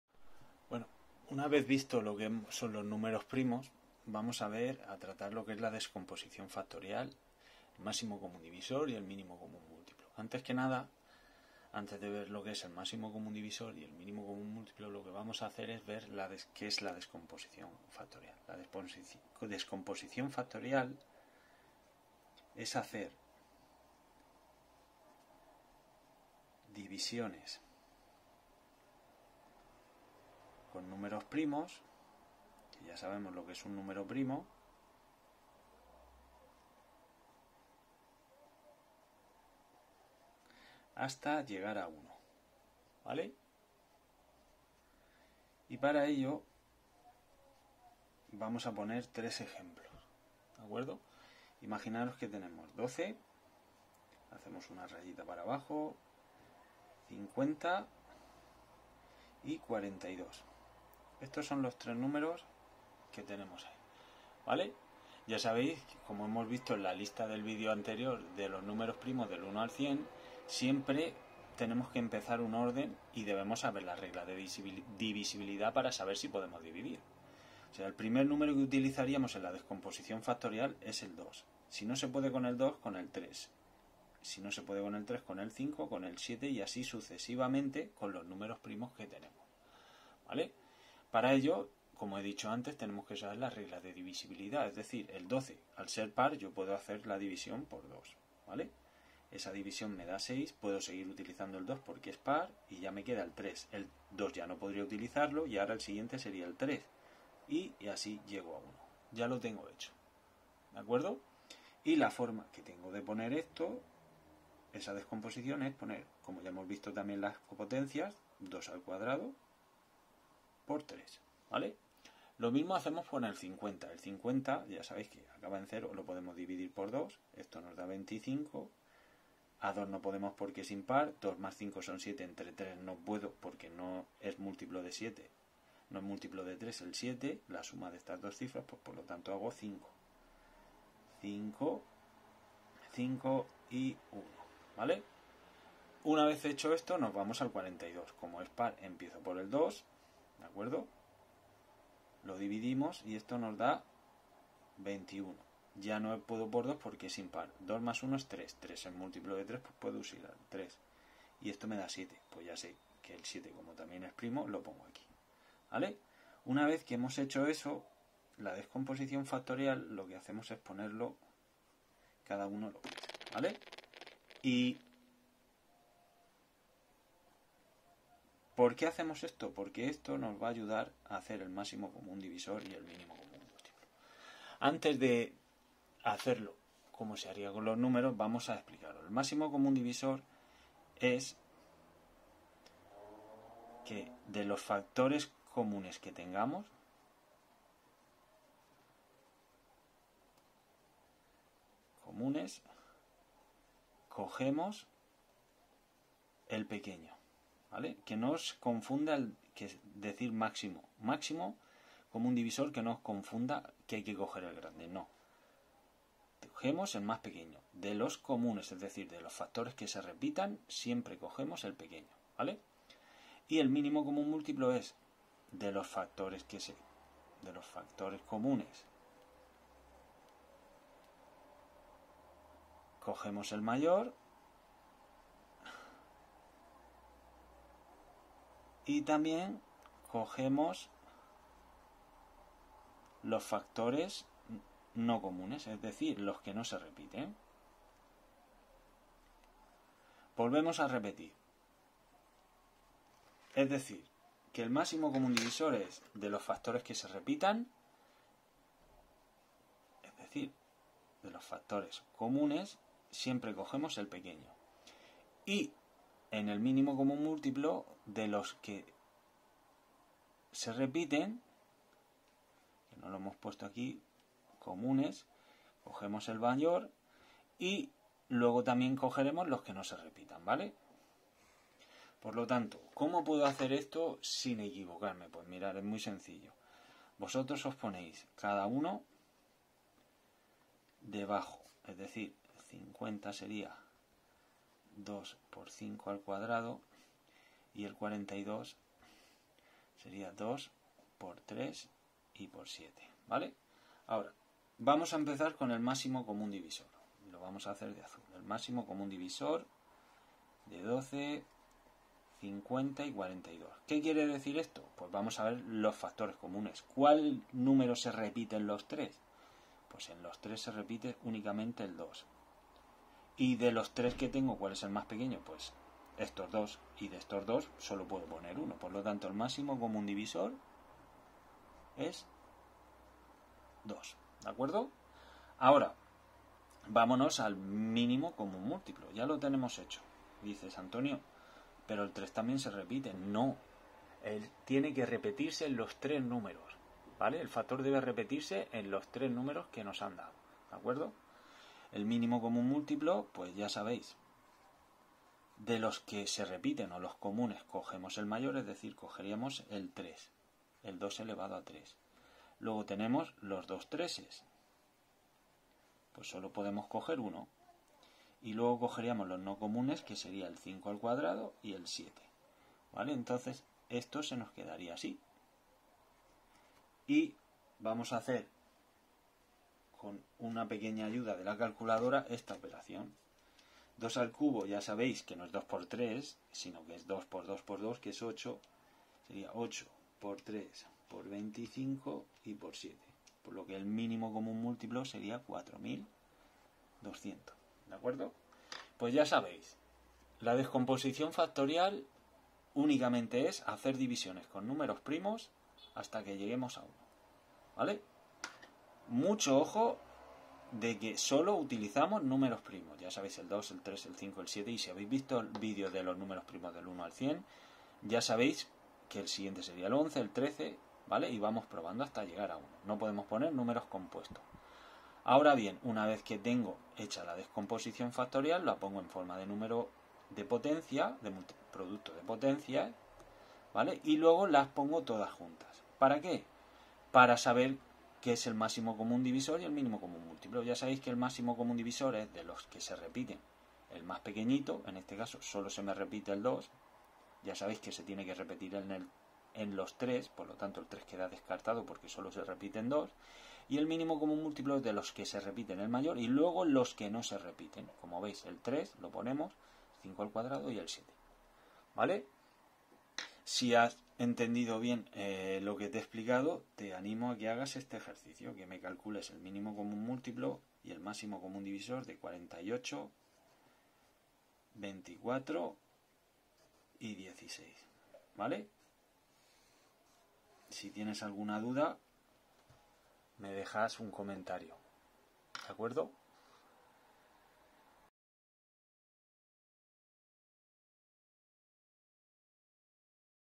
Descomposición factorial, MCD, mcm
Introducción a la descomposición factorial como paso a realizar los cálculos del MCD y mcm de números compuestos.